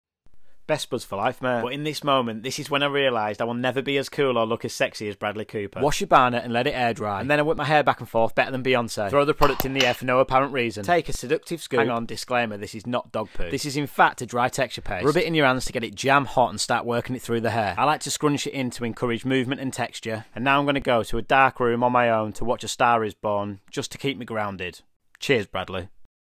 How to style your hair like Bradley Cooper
This is a how to tutorial on how to style your hair like Bradley Cooper